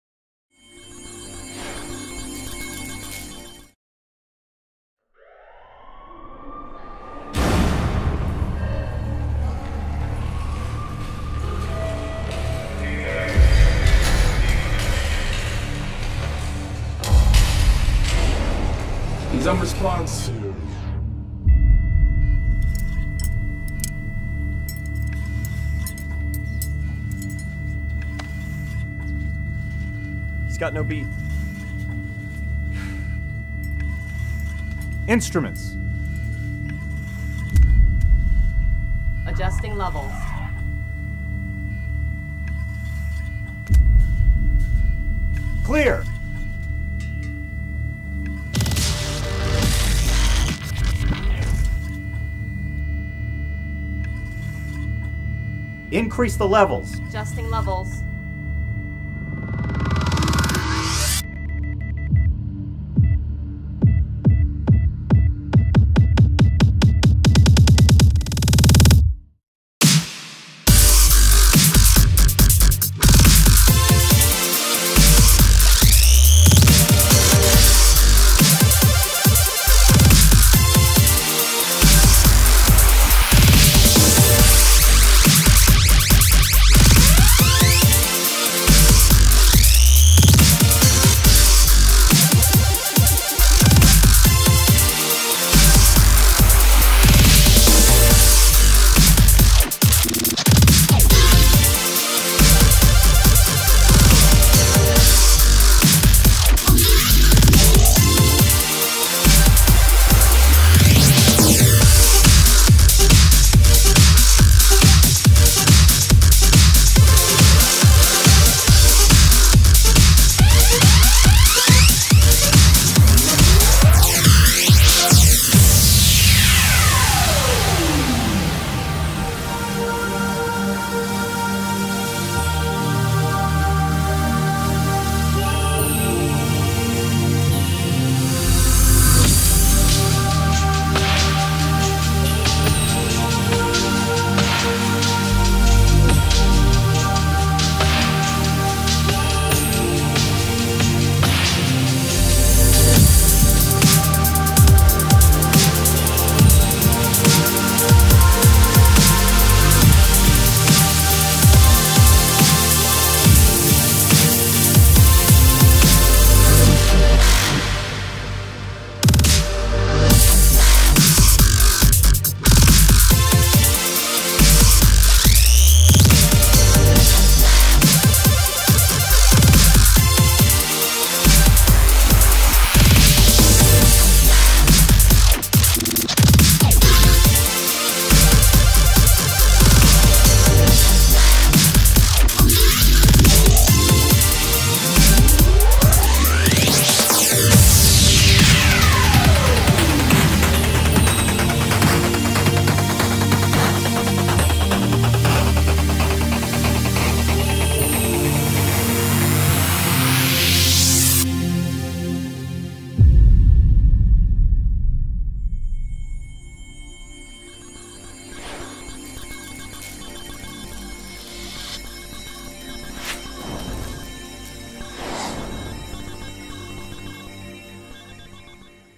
Audio-Technica I Audio 911 I Dubstep
Delivering incredibly full bass and amazing clarity, Audio-Technica's Solid Bass headphones (ATH-WS55) are seen here powering dubstep phenomenon Marquese Scott back to life.  Even the nurse can be seen using the in-ear version of the Solid Bass headphones (ATH-CKS55) as she increases the levels.

Agency: GeniusRocket Inc.